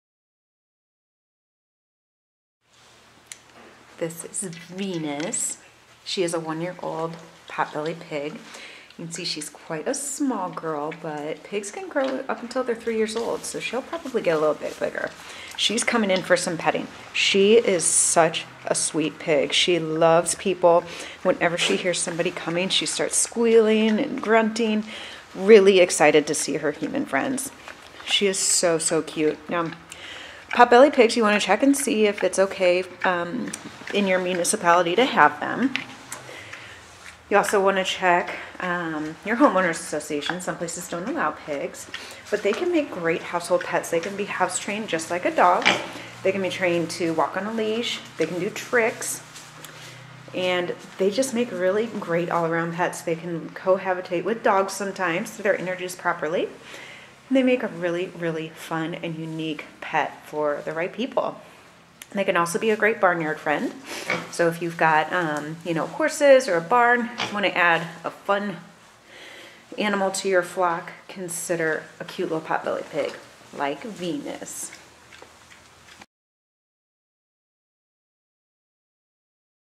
Venus - Available at Longmeadow Rescue Ranch
Venus is an adorable pot bellied pig looking for a wonderful forever home! Venus is a petite girl right now, but she’s just a year old and pigs can grow until they’re three years old! Venus just loves her human companions – whenever someone approaches her enclosure, she starts grunting and squealing with delight. Venus could definitely become a house pig – she had been litter box trained by her previous owner, but she would probably need a refresher in her new home. Pigs are very smart and can learn tricks or even walk on a leash! Be sure to check with your municipality and homeowners association before you adopt to make sure owning a pig is a-ok. Venus is a very sweet and social girl who would love a forever home to call her own!